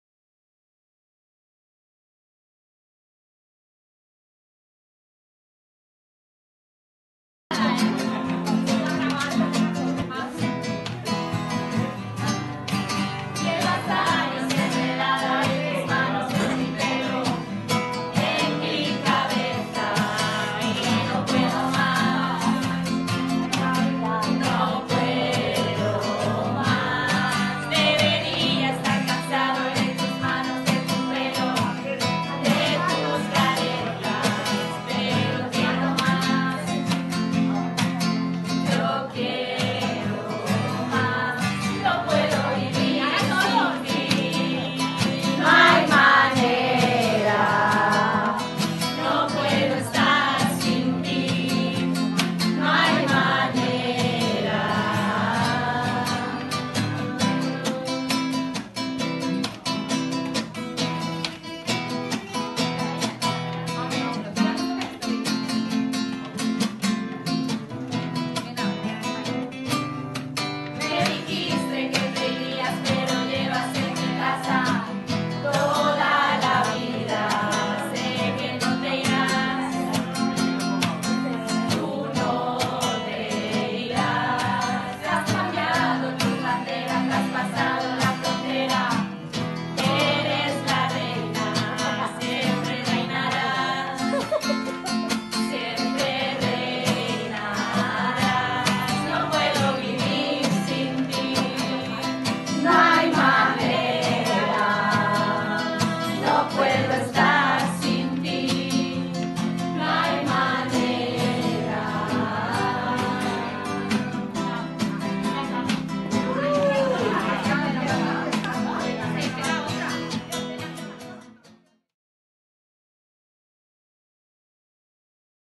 2018 Festa de Nadal al Centre
Un año más, hemos celebrado nuestra tradicional Fiesta de Navidad con actuaciones, bailes, amigo invisible y ¡muchas risas!